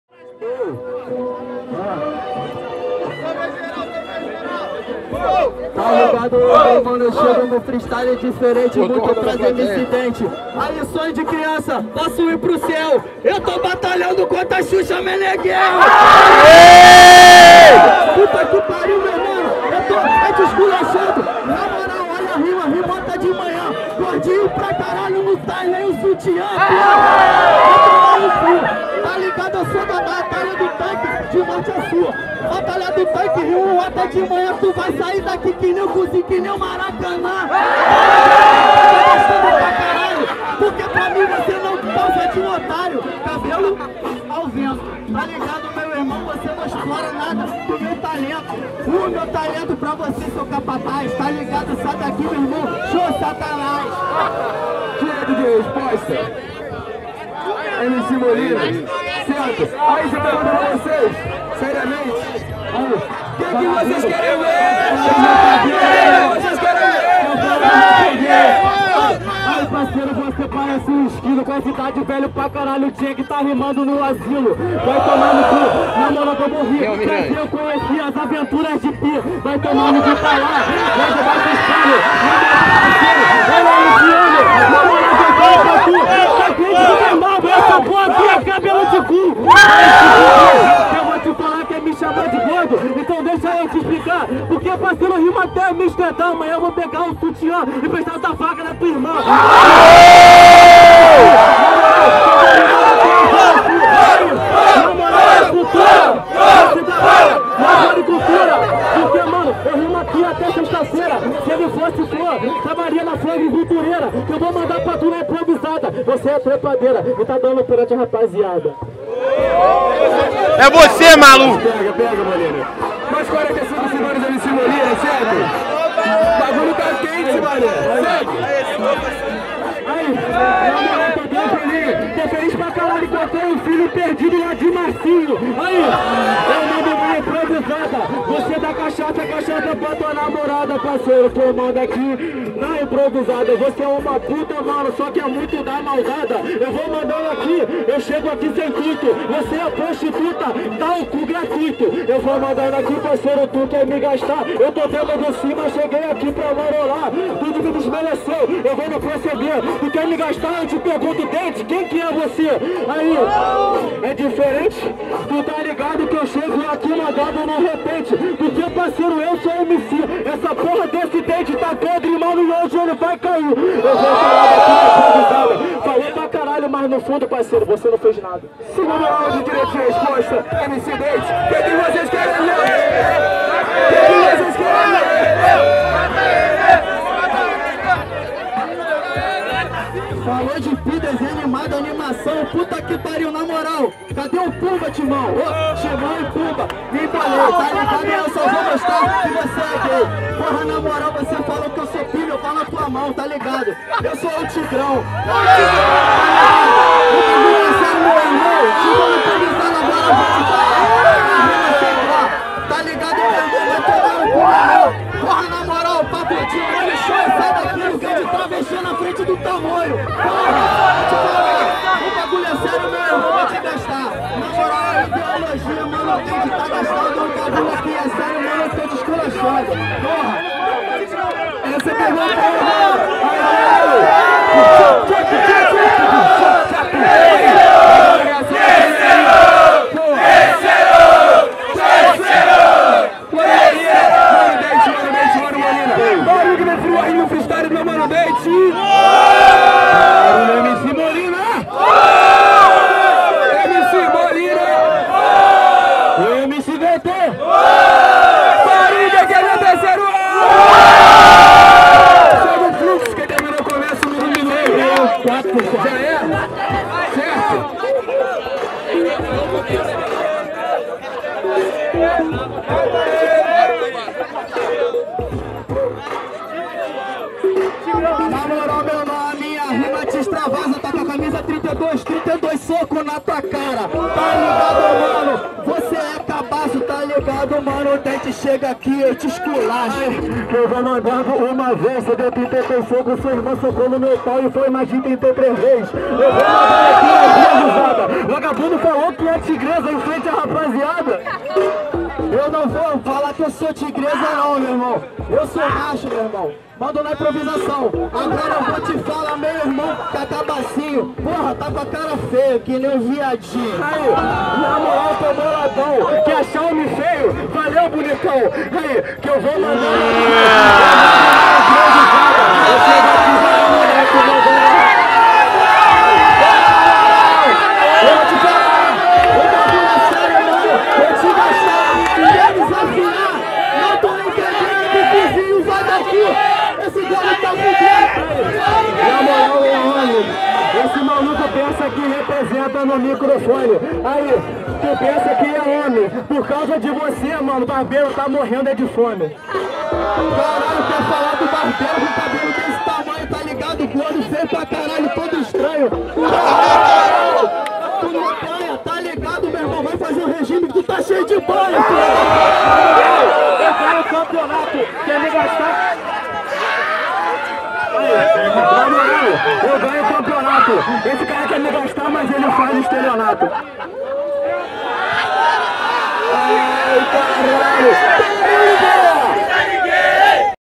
Dent vs Molina - 235º Batalha do Tanque - São Gonçalo - 2017
Vencedor da Batalha:Dent